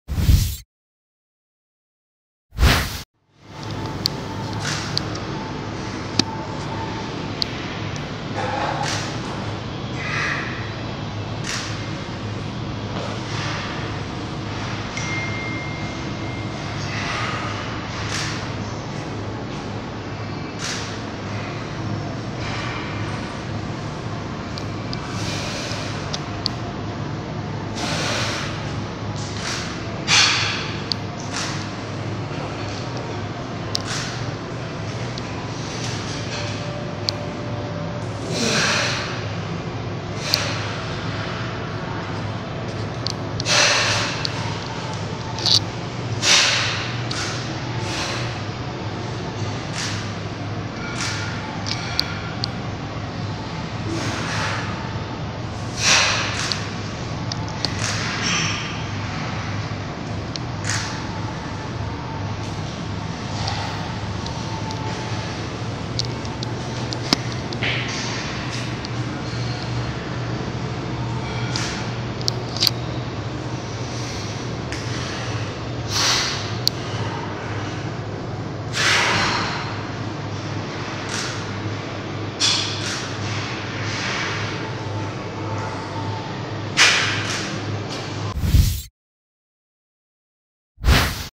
IFBB Pro Evan Centopani Posing Video: 2 Weeks Out From The 2016 Arnold Classic
NPC News caught up with IFBB Pro Evan Centopani at the Montanri Bros Powerhouse Gym In New Haven, Connecuit. Evan is 2 weeks out from the 2016 Arnold Classic. He placed 5th at last years Arnold. Check out these photos from his posing session at the SuperGym . Click here For Evan Centopani Contest Photos.